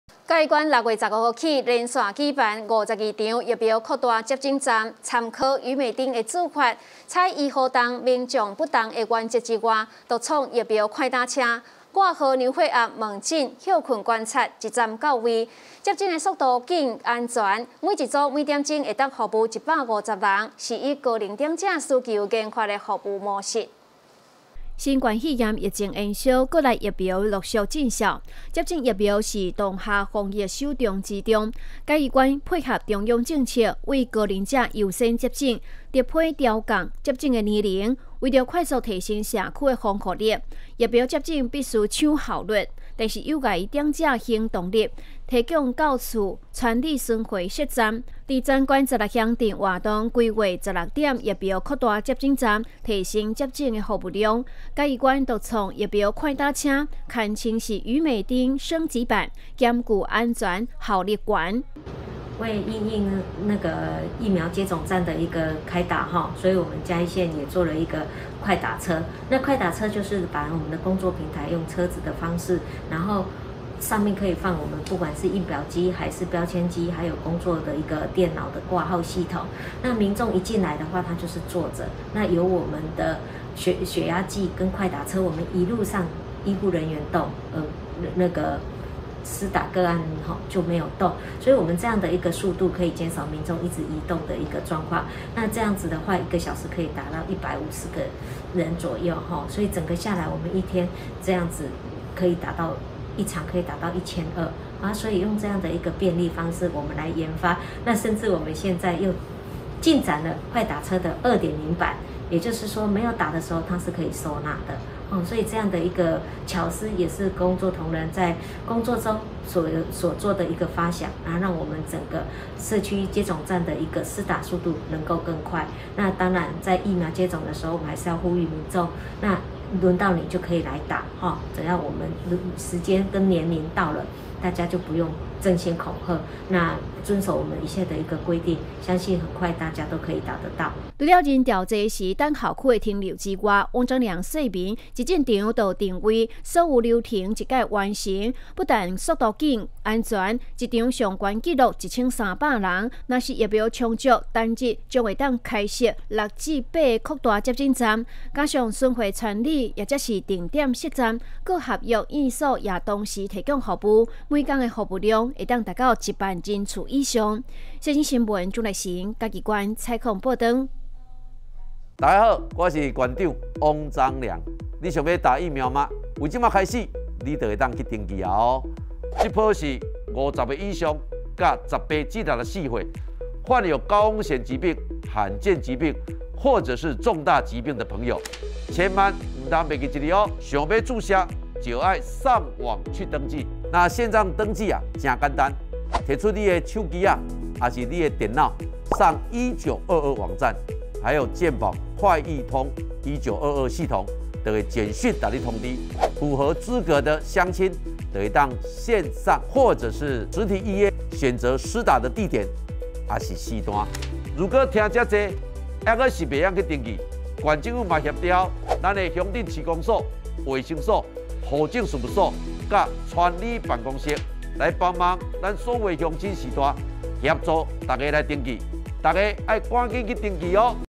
世新新聞 嘉縣獨創疫苗快打車 服務一站到位 單日可萬人接種
110年07月10日 (六) 世新新聞

嘉縣獨創疫苗快打車 服務一站到位 單日可萬人接種

嘉義縣6月15日起連辦理52場疫苗擴大接種站，參考宇美町打法，採醫護動、民眾不動原則外，獨創「疫苗快打車」，掛號、量血壓、問診、休息觀察1站到位，接種速度快、安全，每組每小時可服務150人，是以高齡長者需求研發的服務模式。